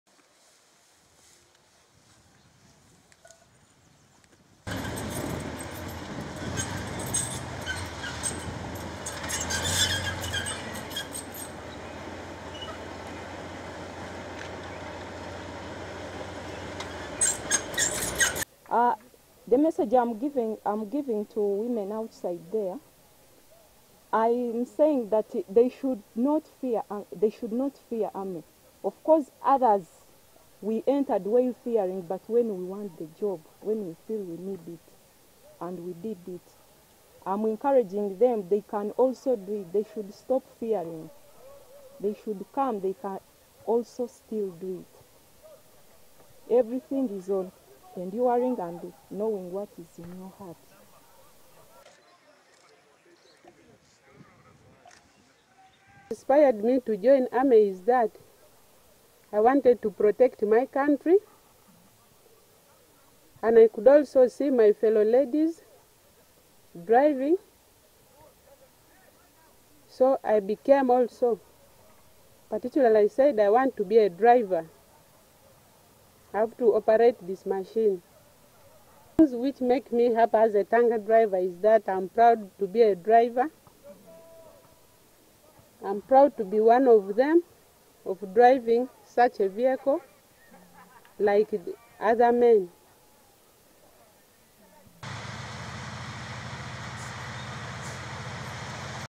Female UPDF armoured tank drivers share experiences
The history of women in the military extends over 4,000 years back throughout a vast number of cultures and nations. From ancient warrior women to the those currently serving in conflict areas like Somalia. Whereas being a soldier is widely viewed by many as a man’s job, this is not the case with Lance Corporal Rita Ayebare and Lance Corpora Alice Achom both  enrolled in the armoured division of Uganda peoples defence forces and tank drivers.